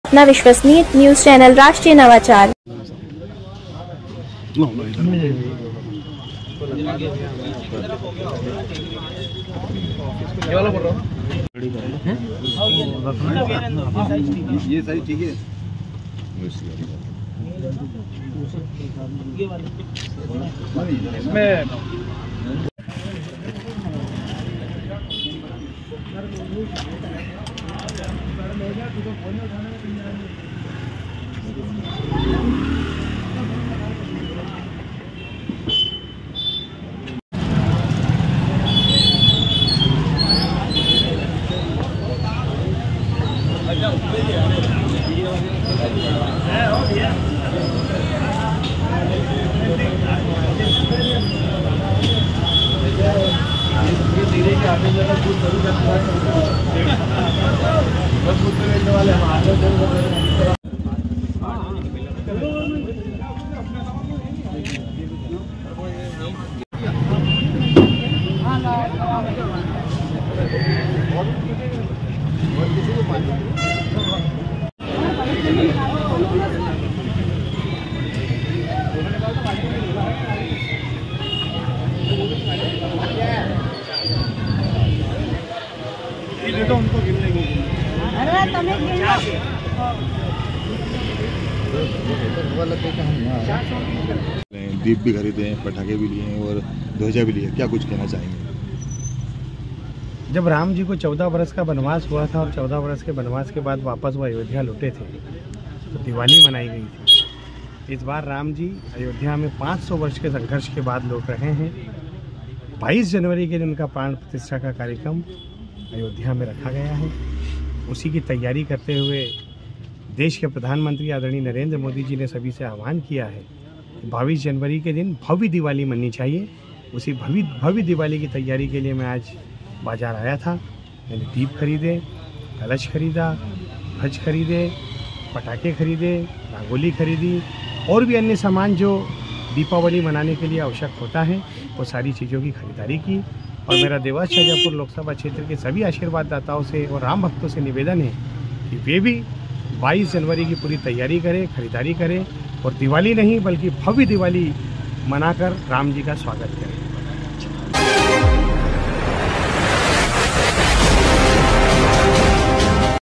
ये क्या माजरा है। सांसद क्यों पहुंचे दीपक, मिठाई, ध्वजा आदि खरीदारी करने बाजार। कौनसी दिवाली आ गई है
22 जनवरी को रामलला के प्राण प्रतिष्ठा अवसर पर दीपावली उत्सव मनाने के लिए सांसद महेंद्रसिंह सोलंकी ने खरीदे बाजार से दीपक, ध्वजा, मिठाई, पटाखे, रोशनी के लिए सीरीज और अन्य सामग्रियां।